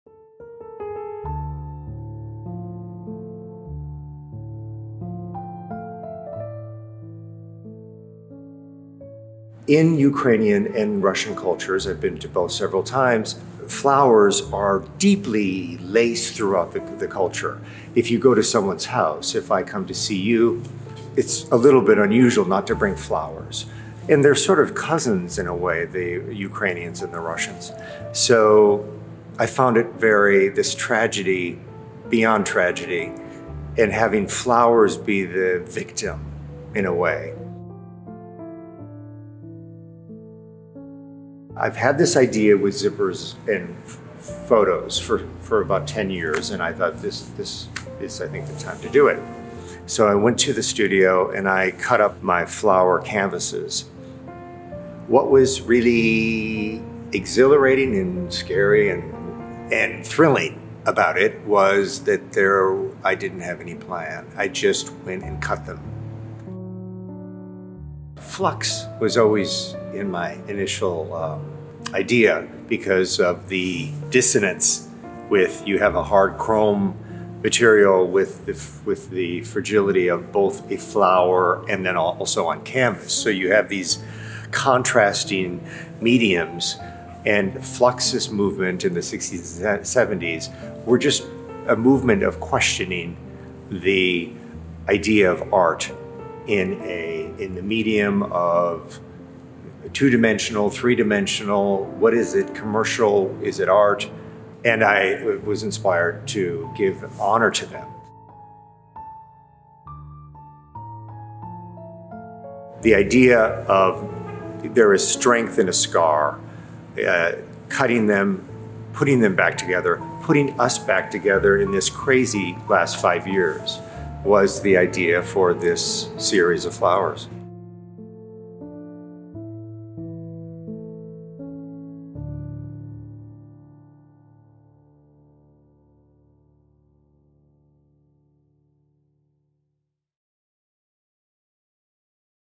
Artist Interview | Paul Solberg
Known for his ethereal flower portraits and refined use of light, Paul Solberg unveils 31 artworks that examine the cyclical nature of humankind’s relationship with power and destruction and the unifying strength in the scars left behind.

Reflecting on his personal experience photographing the Ukrainian military in 2018 and the significance of flowers in both the Ukrainian and Russian cultures, the fall of Bucha in 2022 caused a profound reaction. Returning to the studio, Solberg cut up his flower portraits on canvas, reconstructing the pieces with a new topography, incorporating the cold chrome of a zipper against the flux of the cotton canvas. The resulting body of work titled FLUXUS manifests the price of self-destruction while also celebrating the symbolism of a scar: a mark of tragedy but also a reminder of triumph and survival.

Video by Chris Fortuna